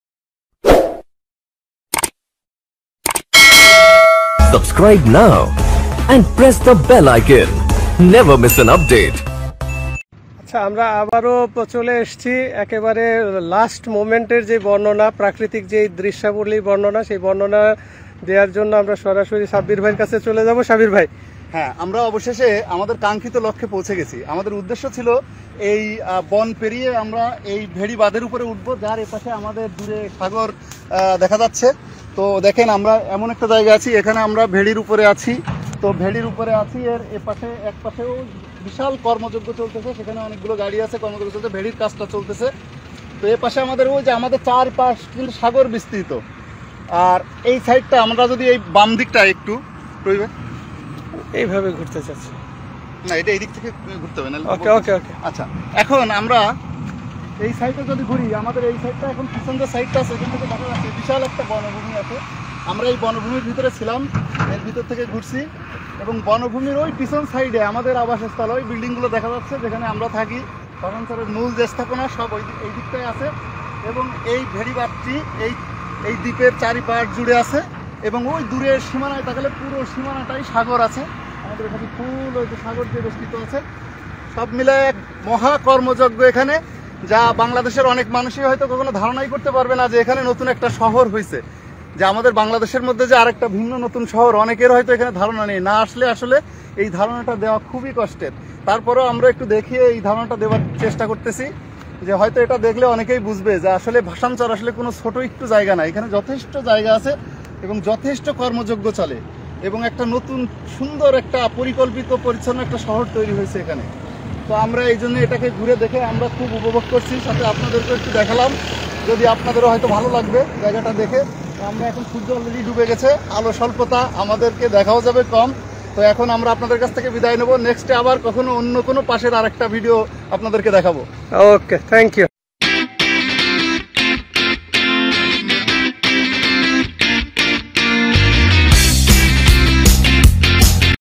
Vlog on Bhasan Char | Part : 01 | Presented by Md. Sabir Hossain | Omiyo TV
Vlog on Bhasan Char
Part : 01
Presented by Md. Sabir Hossain

Some natural beauty senery in Bhasan Char in few some moments for enjoying by my self.!
Nature of beauty by Omiyo TV
A littile bite of joys here for you !